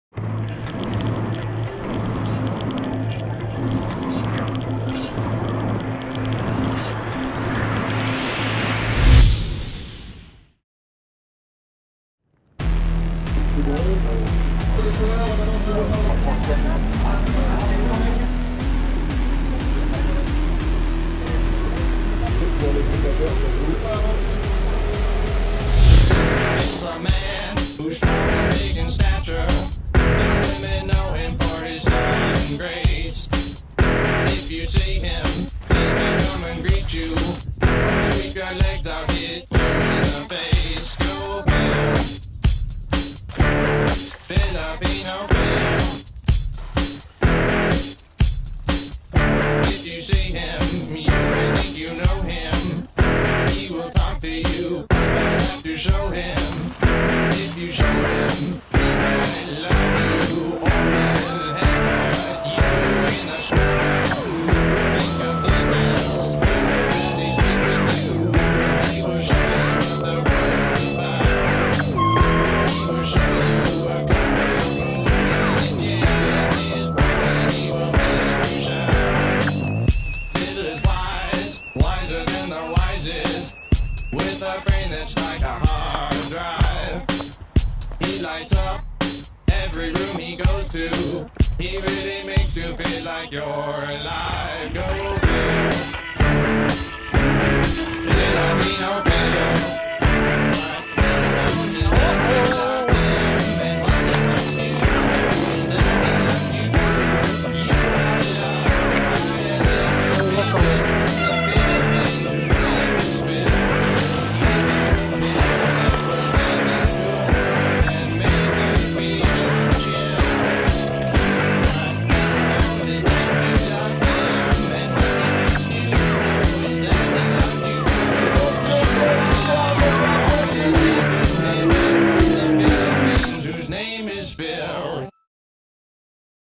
Etape 4 - Les Arves - Transmaurienne-Sybelles 2011
4ème étape entre Saint Jean d'Arves et Saint Sorlin d'Arves.
Dimanche 7 août - Transmaurienne-Sybelles 2011

Rendez-vous du 1er au 5 août 2012 pour la 24ème édition du plus grand raid VTT d'Europe !